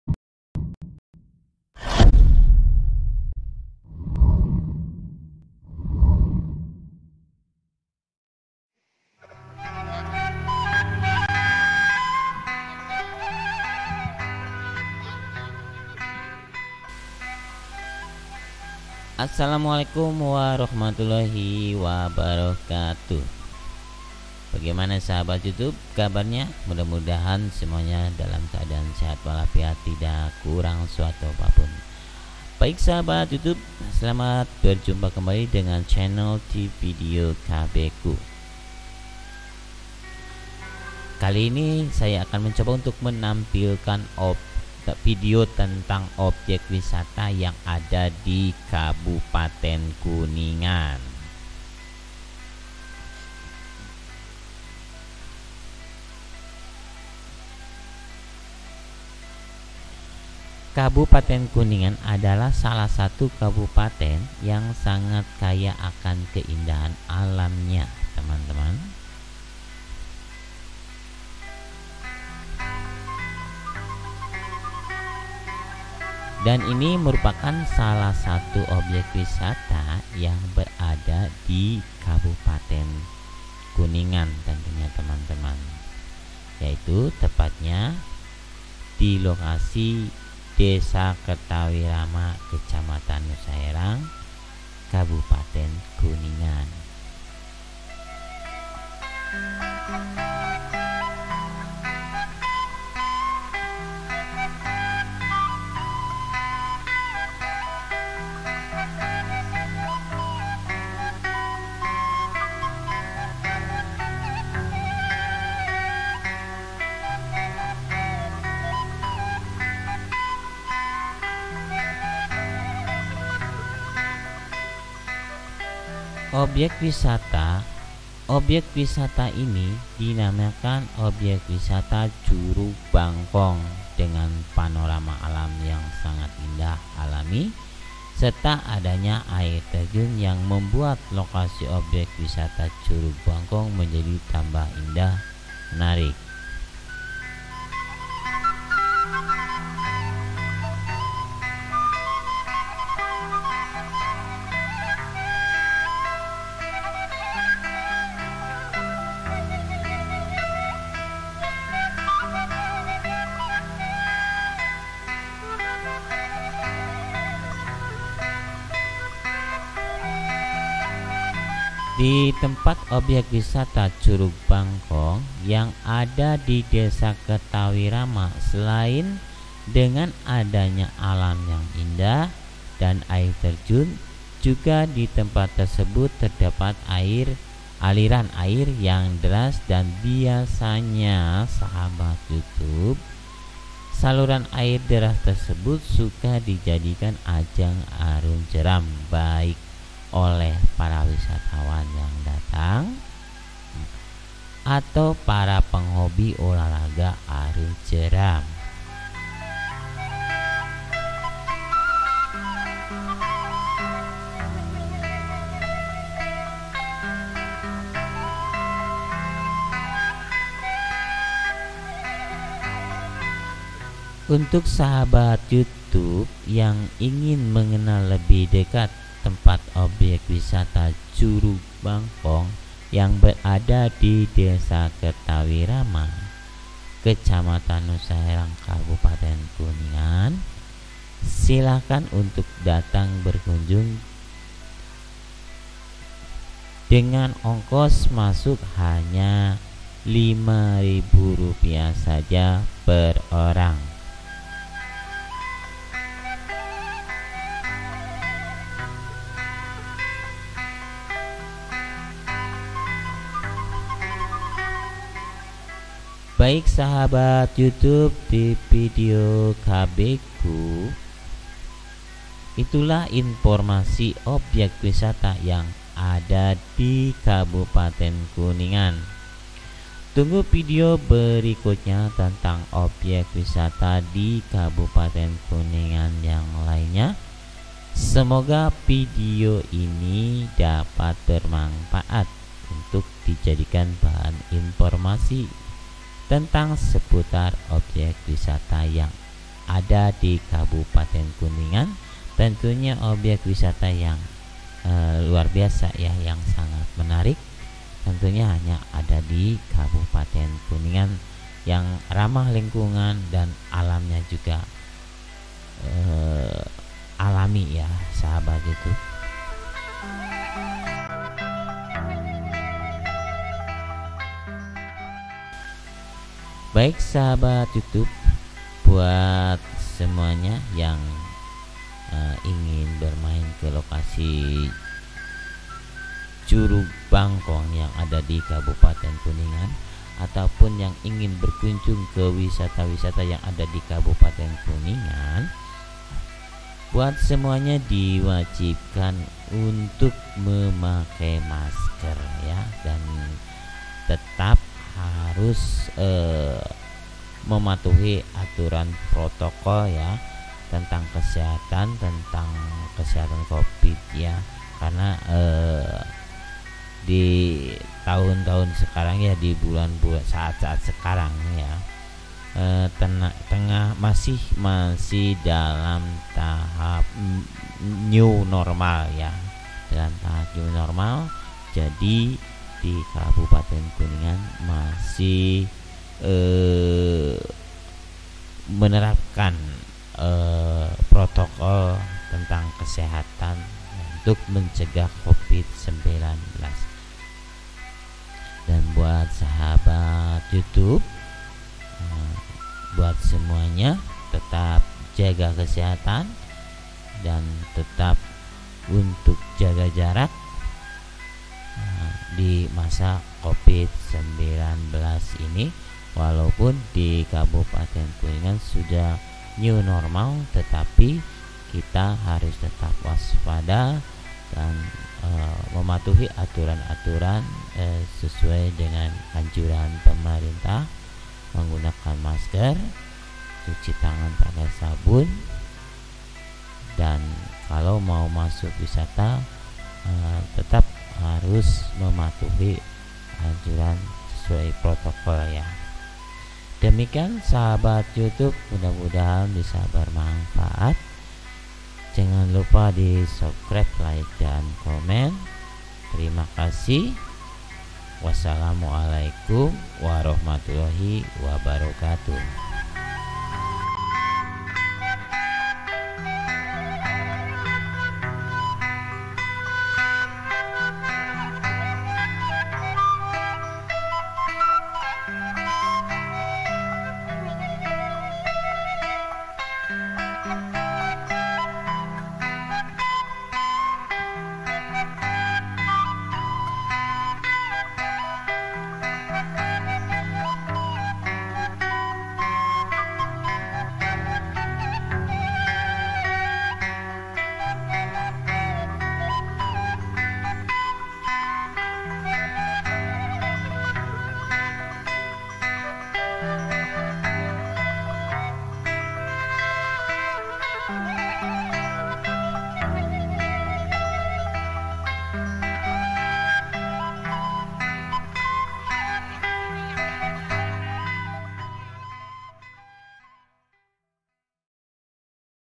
Obyek Wisata Curug Bangkong Kertawirama Nusaherang Kuningan bikin betah sejuk almnnya
Liburan Hiburan menikmati alam indah obyek Wisata Curug Bangkong